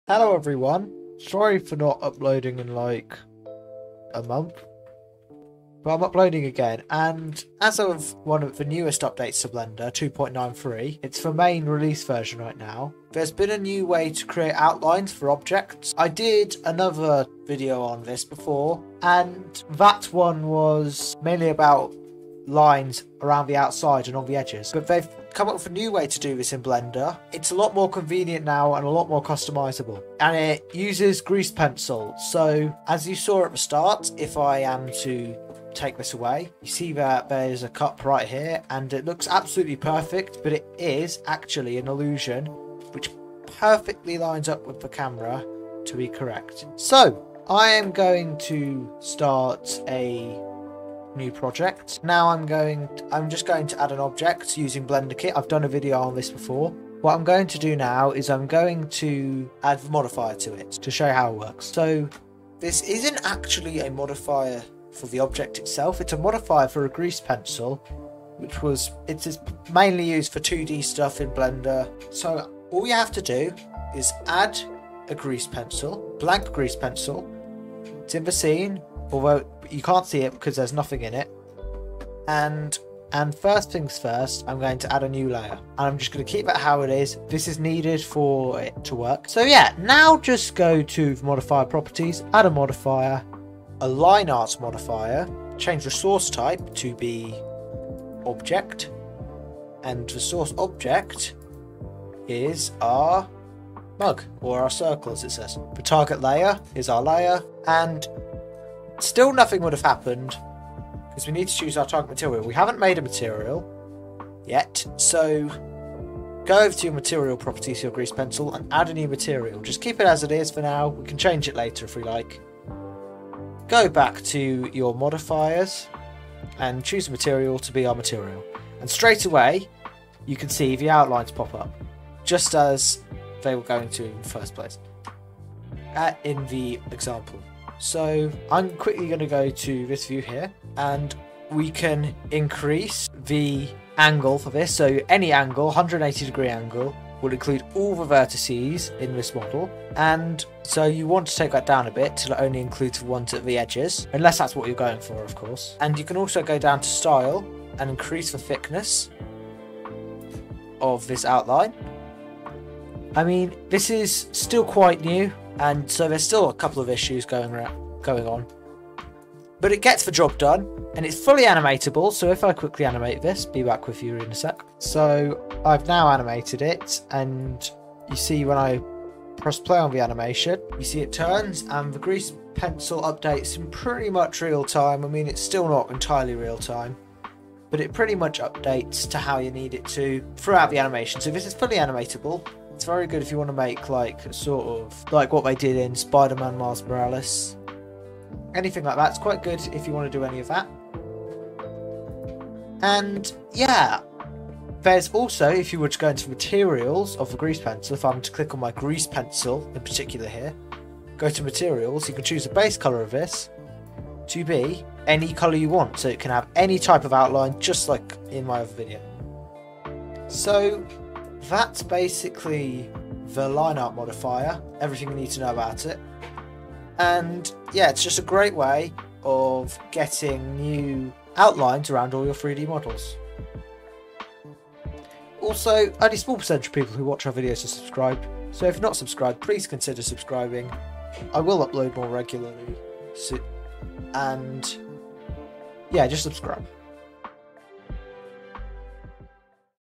Get Grease Pencil Outlines Around Your Model Blender 2.93!
Hello everyone

Welcome to Get grease Pencil outlines around your model in blender 2.93! I would really appreciate it if you liked and subbed as these videos are hard to make. thanks!